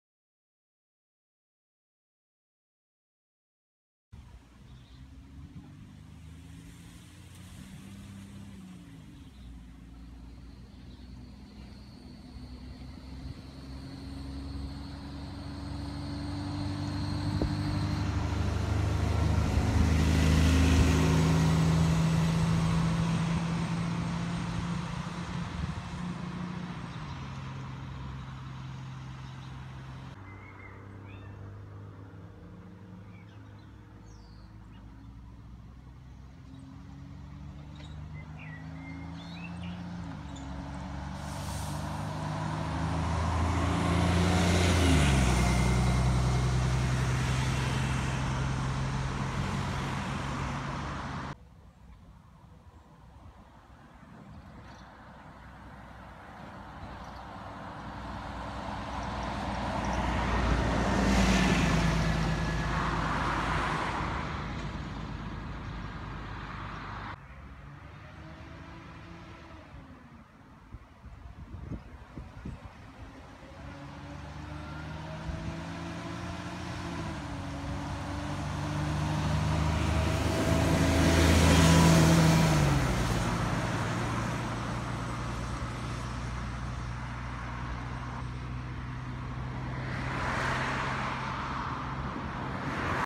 All four of Stagecoach Cambridge's 'Poshviros'
Today's video shows all four of Stagecoach Cambridge's 'Poshviros', the ex-Norfolk Green Enviro 400s. These are different from Stagecoach's regular Enviro 400s, these have the face-lifted front, tinted windows, bonded windows, ZF gearboxes (instead of Voith), high backed seats, and best of all, Fake-Wood flooring.

The buses in the order they appear:
SN13 EEA - 10054
SN12 EHM - 10052
SN12 EHO - 10053
SN13 EEB - 10055

Also notice the displays are finally correct and are now the same to the rest of the fleet.
Please do not use any of my photos or videos without permission, and finally, I do not do Sub4Sub, so don't ask because I WILL NOT accept.

This video was recorded on an iPhone 6, edited with iMovie, and exported as 1080p.